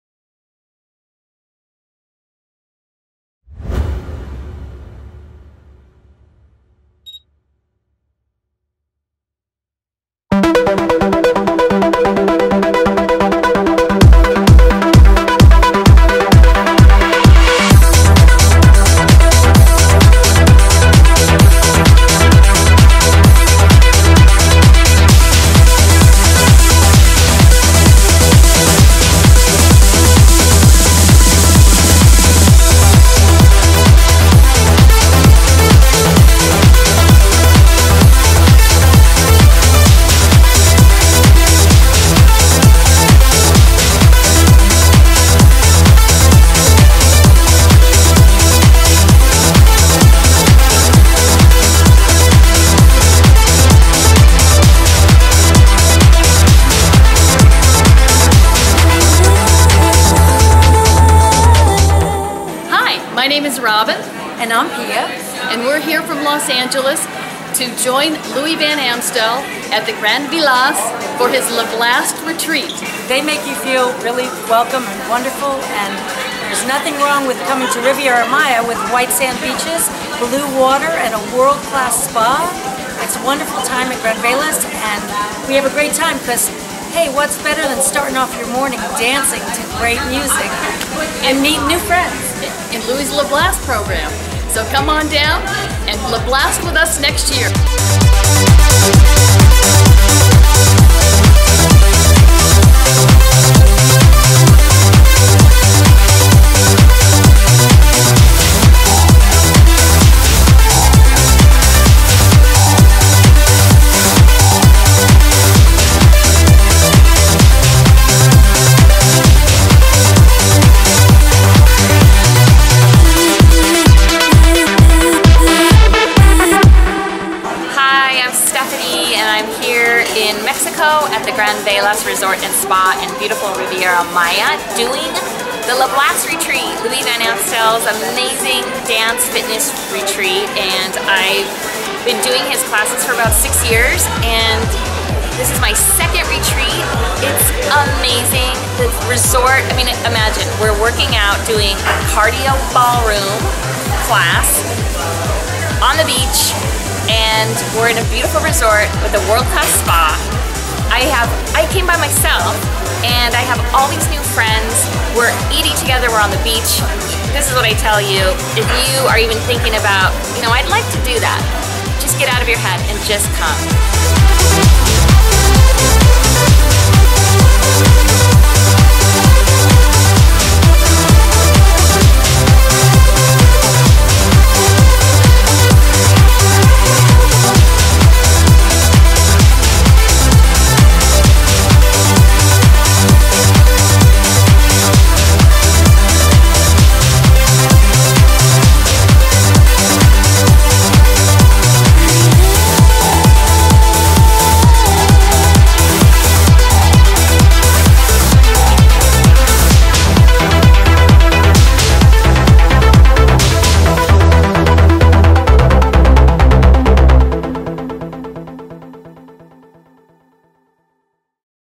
Lablast Retreat - The Experience
with Louis Van Amstel From "Dancing with the Stars"

LaBlast is the hottest new dance fitness program based on the dances seen on 'Dancing with the Stars', it's completely partner, and uses music from every era, every genre, end every artist! No experience needed.